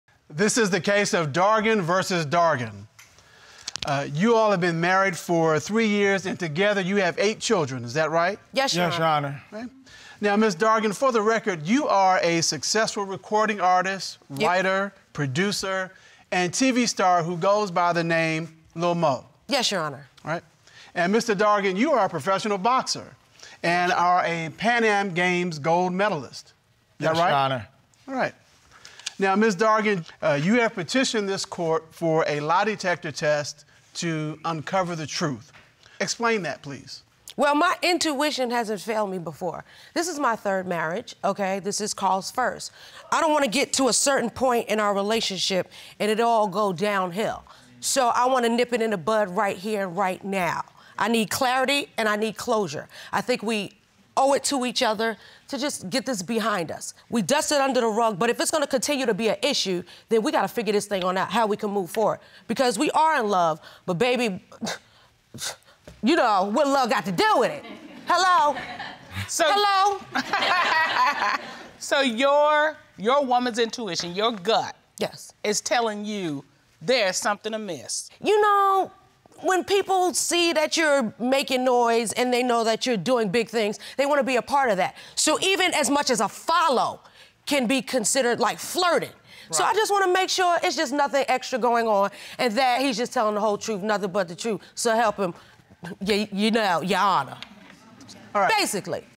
LIL MO OF "LOVE AND HIP HOP NEW YORK" IS IN COURT WITH HER THIRD HUSBAND, KARL DARGAN
Dargan v Dargan
RECORDING ARTIST AND STAR OF LOVE AND HIP HOP NEW YORK, LIL MO, BRINGS HER HUSBAND KARL DARGAN TO COURT TO GET TO THE BOTTOM OF THE CHEATING SCANDALS THAT ARE CIRCULATING IN THE MEDIA.